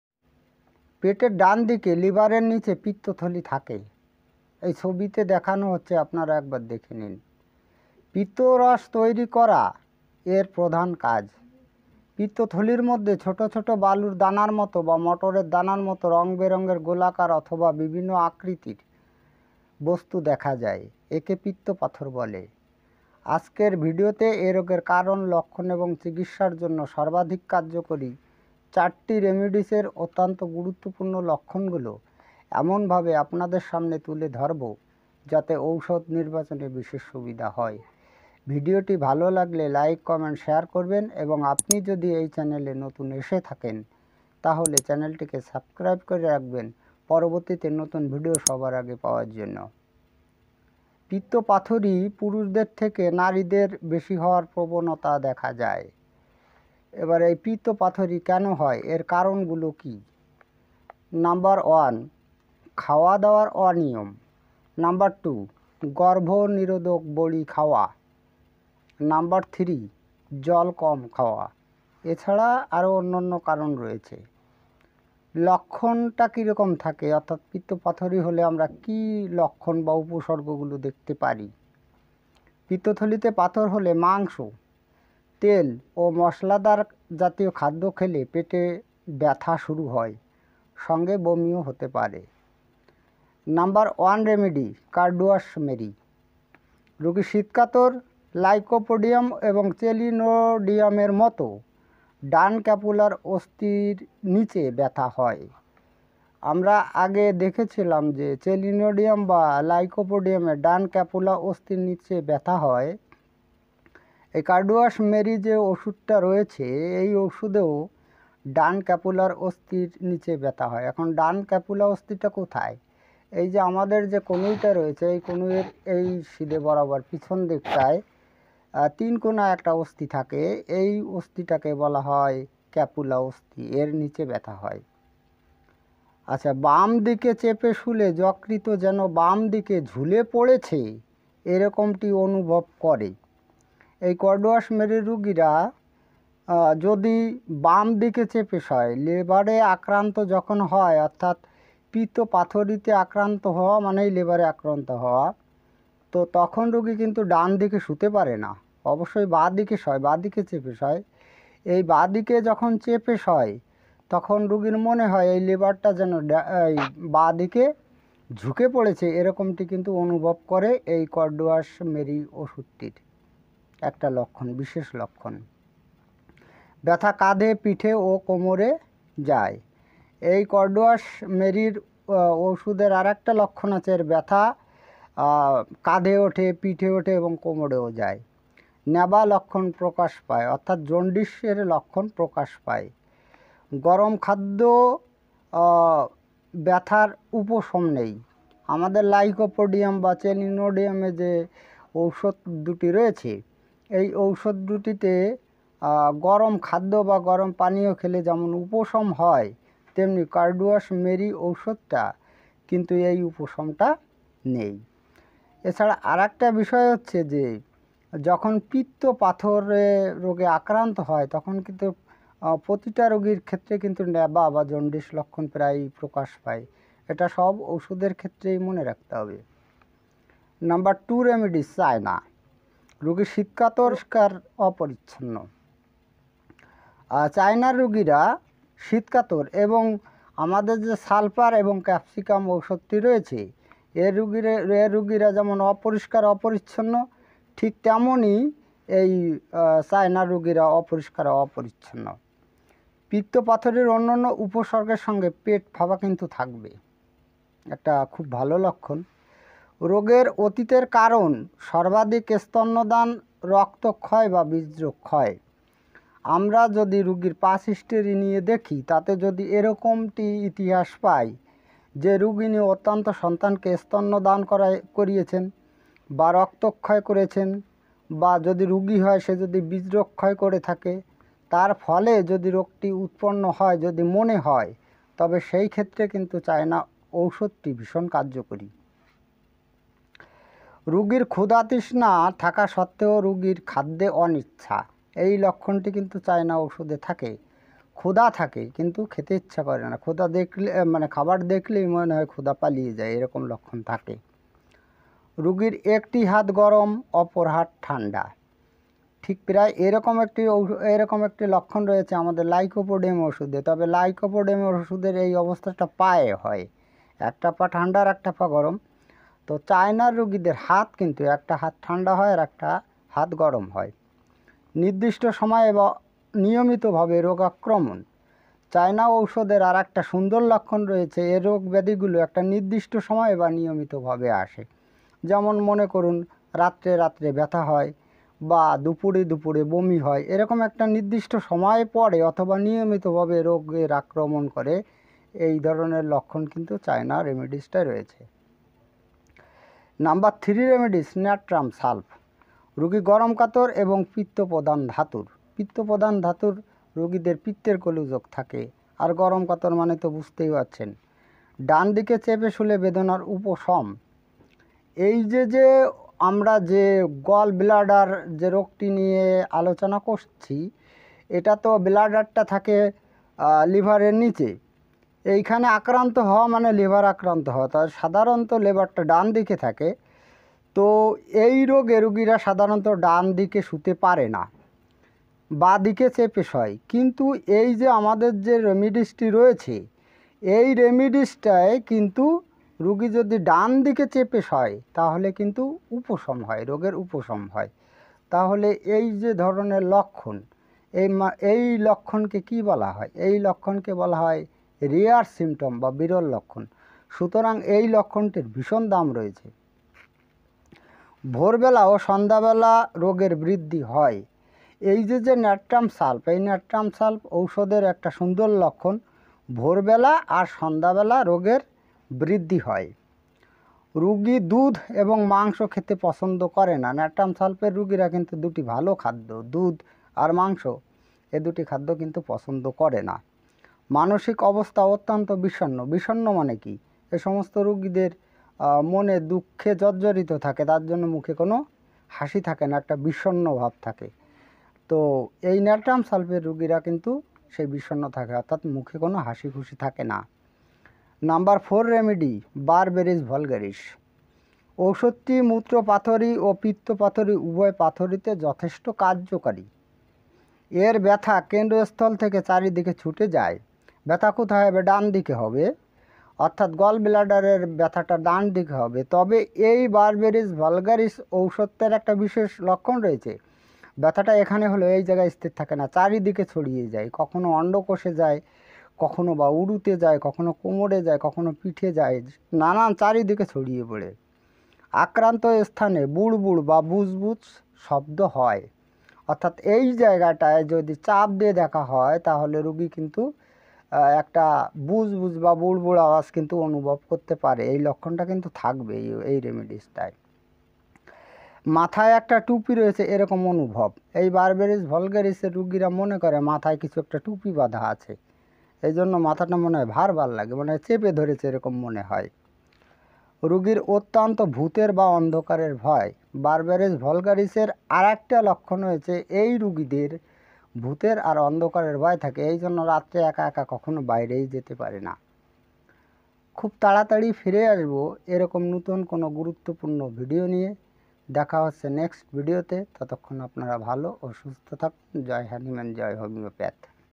(69) 'পিত্ত পাথরির' হোমিও চিকিৎসা* কারণ * লক্ষণ -Homoeo treatment of 'Gall bladder stone'...
Homoeo treatment of 'Gall bladder stone'*symptoms*causes & its most important symptoms 4 remedies.